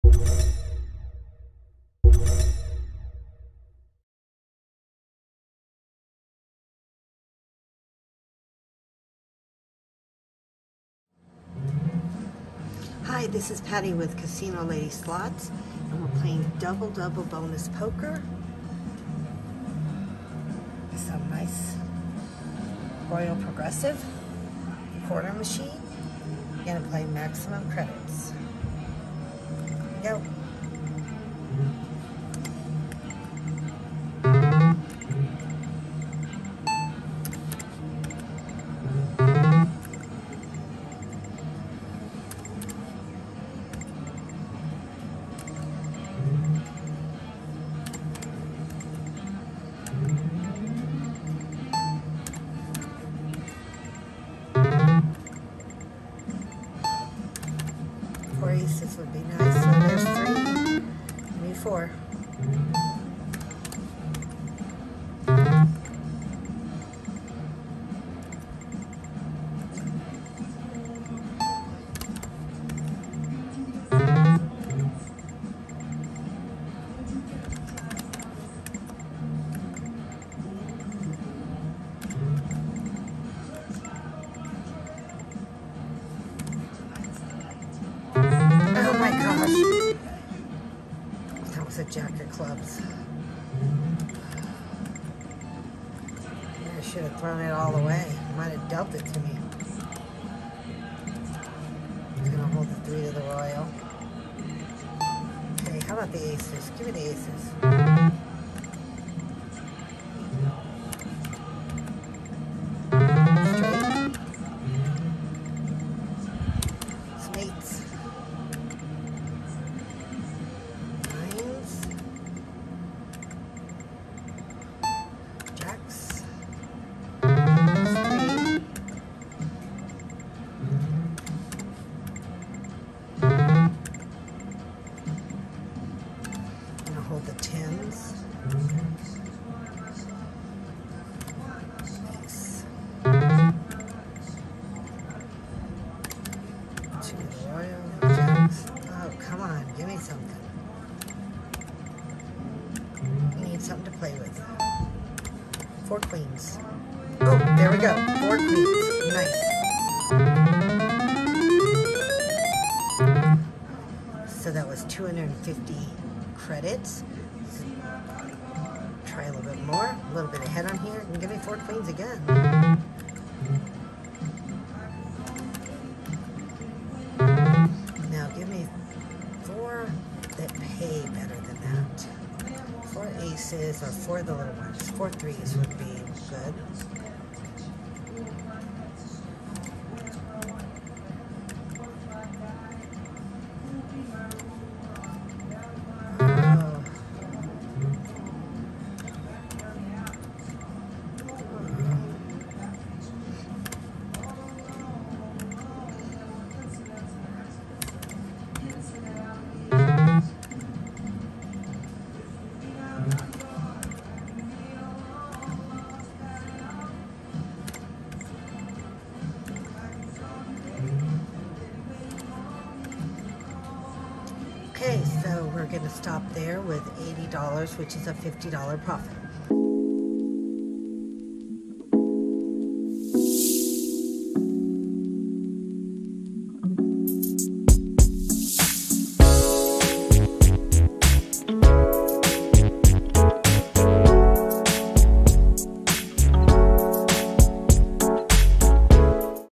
Double Double Bonus Poker Video Poker Session. Nice Profit!
I am playing Double Double Bonus Poker at the Midnight Rose Casino in Cripple Creek Colorado!